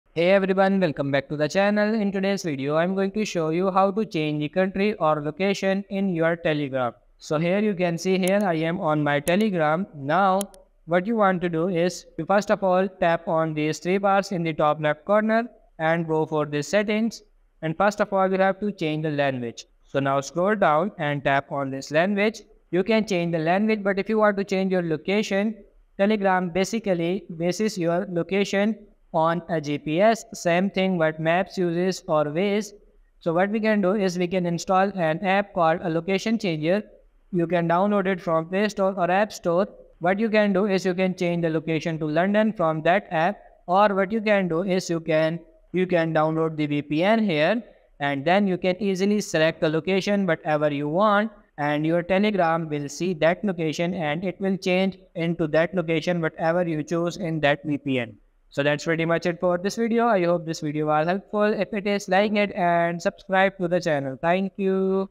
How to Change Location on Telegram - Easy 2024
If you want to Find How to Change Location on Telegram but don't know how to do it, then this tutorial is for you!

In this tutorial, I'm going to show you How to Change Location on Telegram.